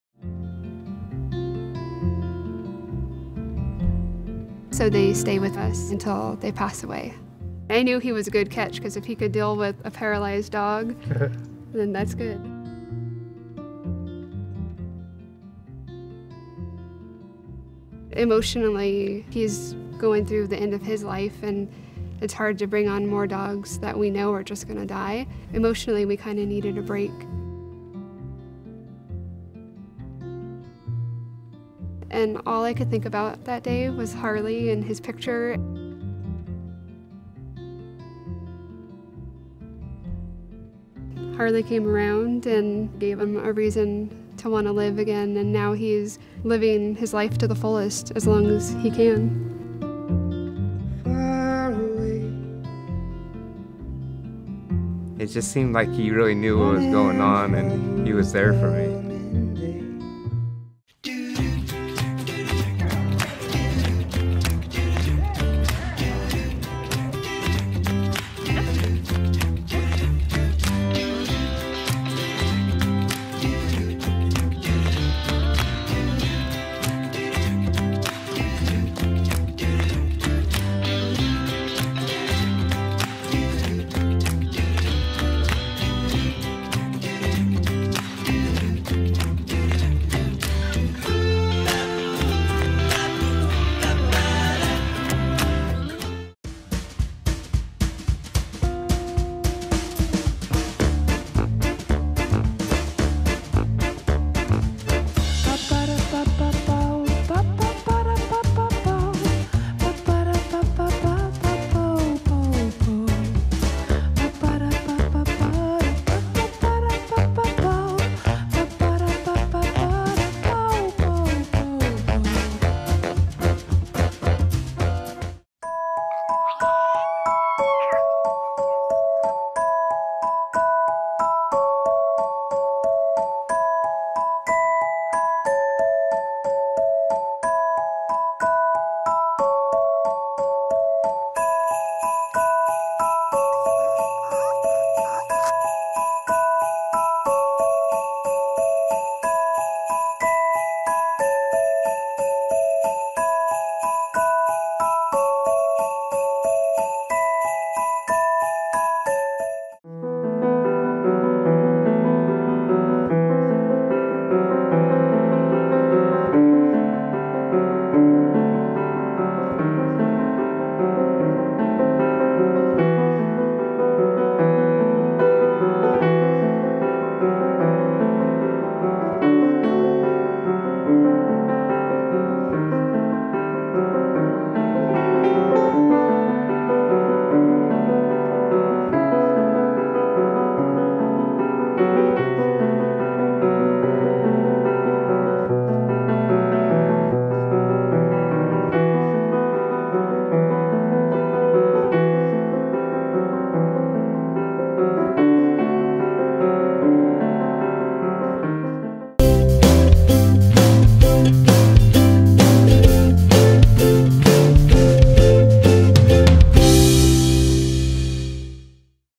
Dog Helps Dad Through Cancer Treatment: Best Animal Videos | The Dodo Daily
Best Animal Videos include: Senior Dog Helps His Dad Through Cancer Treatment; Pig Works at Airport; Cat Who Got Sick In A Pet Store Loves Her New Family; Rescue Pup With Special Needs Is Becoming A Therapy Dog; Dolphins Forced To Perform In Traveling Circuses

Senior Dog Helps His Dad Through Cancer Treatment
Special thanks to Molly Dog Films for this incredible footage!

Pig Works at Airport
Keep up with the adventures of Lilou the therapy pig on Instagram Learn more about San Fransisco SPCA’s Wag Brigade

Cat Who Got Sick In A Pet Store Loves Her New Family
To help rescue other cats like Moet, please support Oman Rescue

Rescue Pup With Special Needs Is Becoming A Therapy Dog
Special thanks to Fred’s Friends Rescue for giving Albertino Pupperino a second chance Follow them on Instagram for updates on his progress

Dolphins Forced To Perform In Traveling Circuses
To sign a petiton calling for the end of the Dolphin Traveling Circus, please visit Ric O'Barry's Dolphin Project